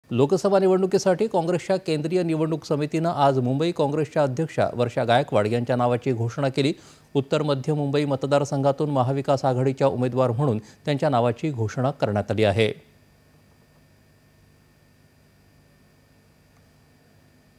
उत्तर मध्य मुंबई मतदार संघातून महाविकास आघाडीच्या उमेदवार म्हणून वर्षा गायकवाड यांच्या नावाची घोषणा.
दि. २५.०४.२०२४ :

लोकसभा निवडणुकीसाठी काँग्रेसच्या केंद्रीय निवडणुक समितीनं आज मुंबई काँग्रेसच्या अध्यक्षा वर्षा गायकवाड यांच्या नावाची घोषणा केली. उत्तर मध्य मुंबई मतदार संघातून महाविकास आघाडीच्या उमेदवार म्हणून त्यांच्या नावाची घोषणा करण्यात आली आहे.

8.30am | 11.00am | 1.00pm | 4.30pm | 7.00pm | 9.30pm